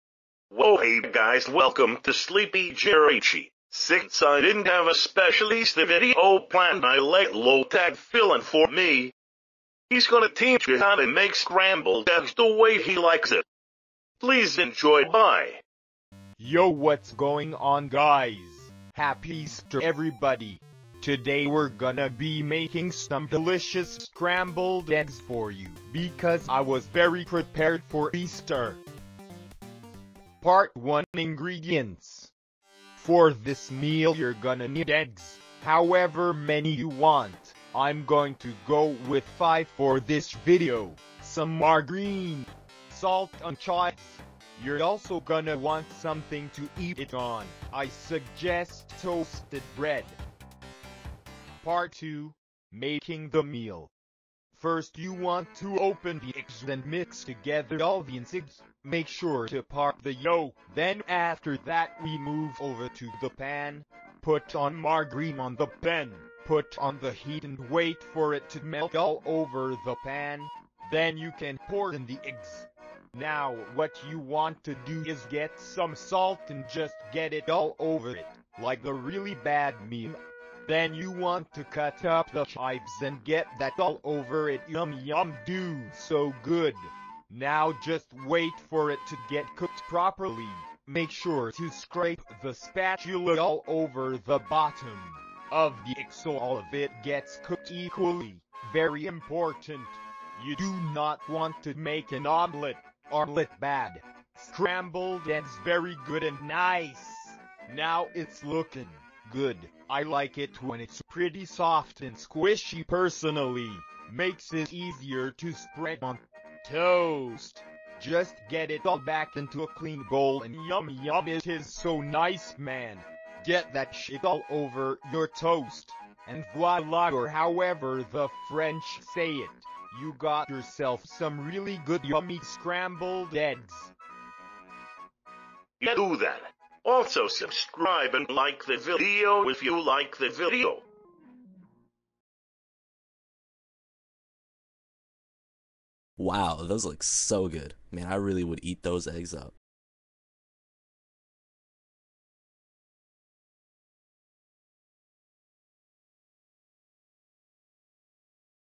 How To Make Scrambled Eggs (w/
Hello hope you all had a fantastic Easter. Need something to do with all those eggs you found from rabbits? You can now learn how to make your own scrambled eggs featuring Lotad_ ! Thanks Lotad.

Shirts available here!